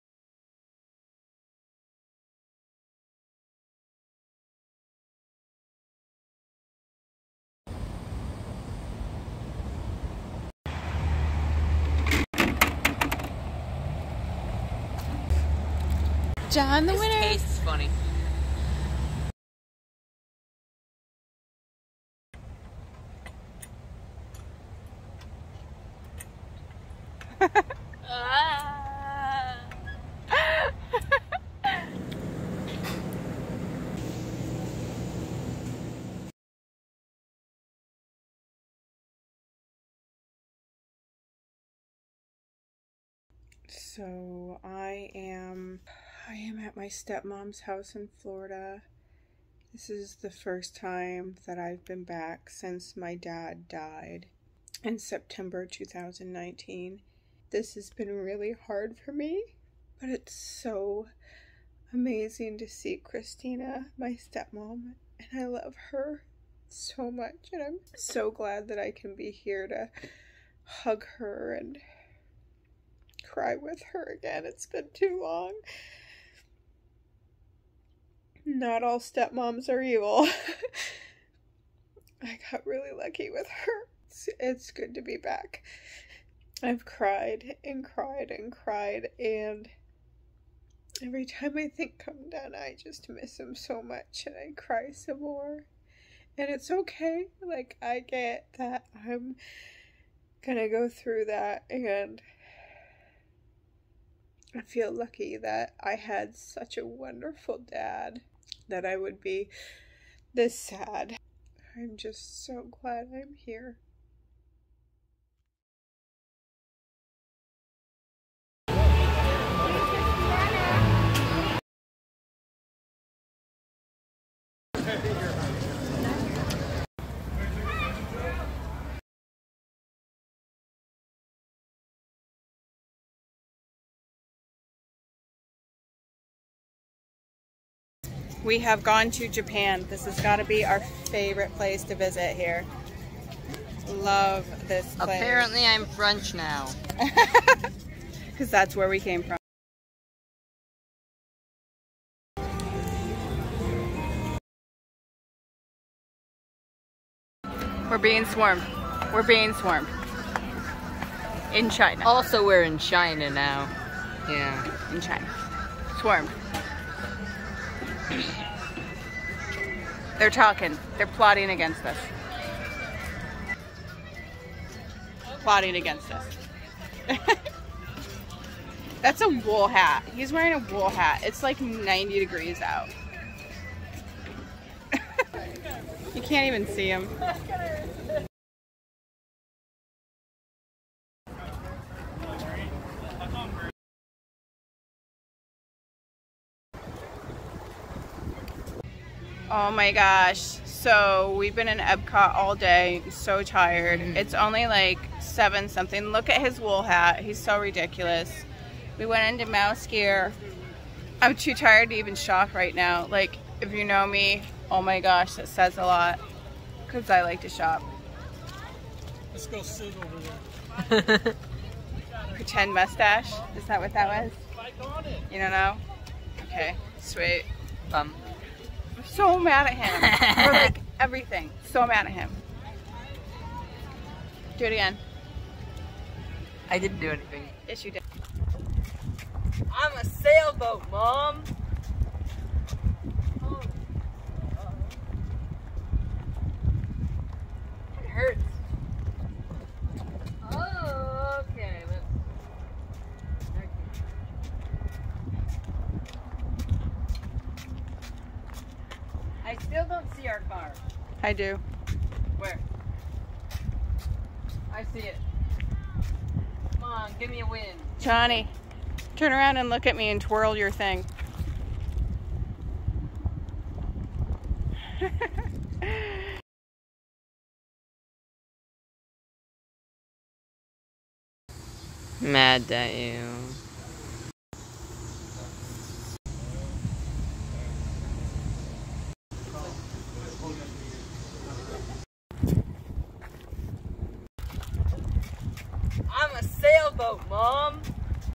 Ep 3 Georgia to Florida
Sara and Jon travel from North Carolina to Georgia and on to Florida. This video shows the first day in Orlando and a day at Disney's EPCOT.